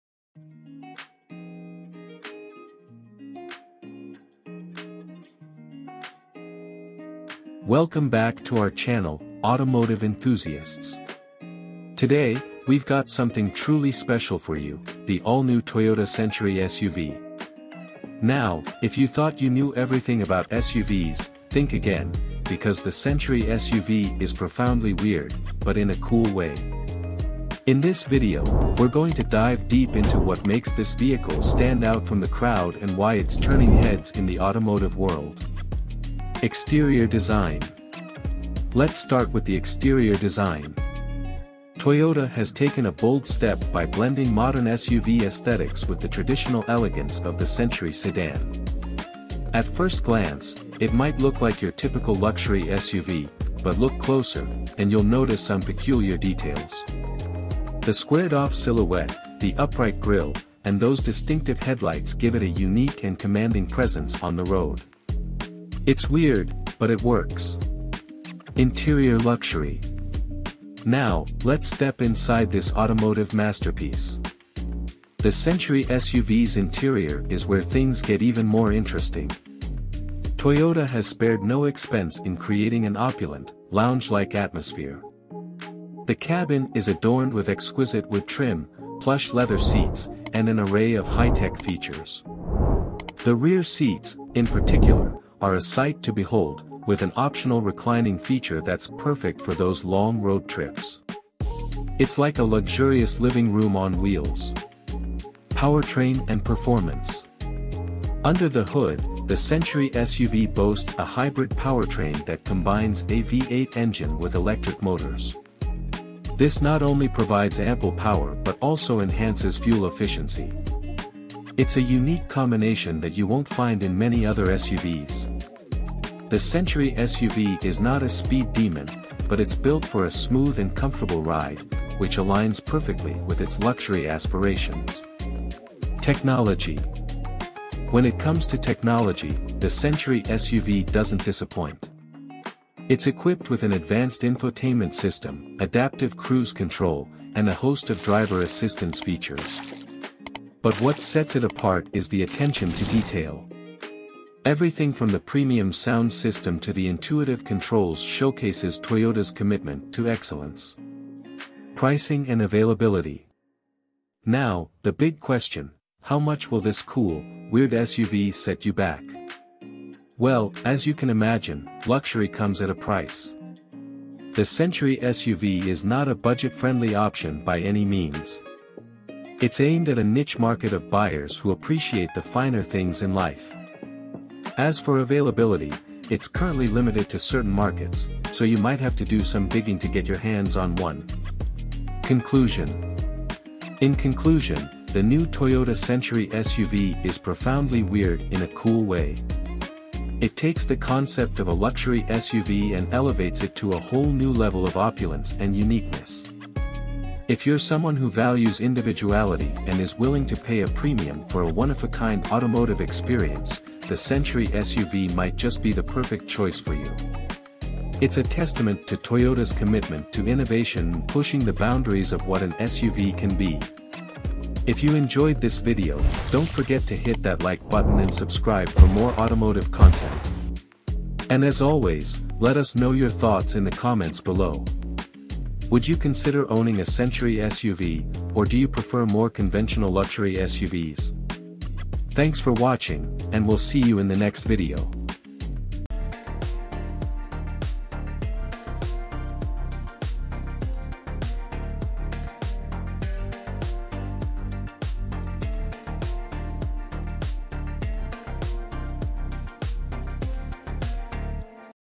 New Toyota Century SUV Is Profoundly Weird in a Cool Way
It's a new take on a conservative Japanese tradition that happens to look like an unlicensed Rolls-Royce Cullinan copy.

Today, we've got something truly special for you – the all-new Toyota Century SUV. Now, if you thought you knew everything about SUVs, think again, because the Century SUV is profoundly weird, but in a cool way. In this video, we're going to dive deep into what makes this vehicle stand out from the crowd and why it's turning heads in the automotive world.

[EXTERIOR DESIGN]
Let's start with the exterior design. Toyota has taken a bold step by blending modern SUV aesthetics with the traditional elegance of the Century sedan. At first glance, it might look like your typical luxury SUV, but look closer, and you'll notice some peculiar details. The squared-off silhouette, the upright grille, and those distinctive headlights give it a unique and commanding presence on the road. It's weird, but it works!

[INTERIOR LUXURY]
Now, let's step inside this automotive masterpiece. The Century SUV's interior is where things get even more interesting. Toyota has spared no expense in creating an opulent, lounge-like atmosphere. The cabin is adorned with exquisite wood trim, plush leather seats, and an array of high-tech features. The rear seats, in particular, are a sight to behold, with an optional reclining feature that's perfect for those long road trips. It's like a luxurious living room on wheels.

[POWERTRAIN AND PERFORMANCE]
Under the hood, the Century SUV boasts a hybrid powertrain that combines a V8 engine with electric motors. This not only provides ample power but also enhances fuel efficiency. It's a unique combination that you won't find in many other SUVs. The Century SUV is not a speed demon, but it's built for a smooth and comfortable ride, which aligns perfectly with its luxury aspirations.

[TECHNOLOGY]
When it comes to technology, the Century SUV doesn't disappoint. It's equipped with an advanced infotainment system, adaptive cruise control, and a host of driver assistance features. But what sets it apart is the attention to detail. Everything from the premium sound system to the intuitive controls showcases Toyota's commitment to excellence.

Now, the big question: how much will this cool, weird SUV set you back? Well, as you can imagine, luxury comes at a price. The Century SUV is not a budget-friendly option by any means. It's aimed at a niche market of buyers who appreciate the finer things in life. As for availability, it's currently limited to certain markets, so you might have to do some digging to get your hands on one.

[CONCLUSION]
In conclusion, the new Toyota Century SUV is profoundly weird in a cool way. It takes the concept of a luxury SUV and elevates it to a whole new level of opulence and uniqueness. If you're someone who values individuality and is willing to pay a premium for a one-of-a-kind automotive experience, the Century SUV might just be the perfect choice for you. It's a testament to Toyota's commitment to innovation and pushing the boundaries of what an SUV can be.

Would you consider owning a Century SUV, or do you prefer more conventional luxury SUVs?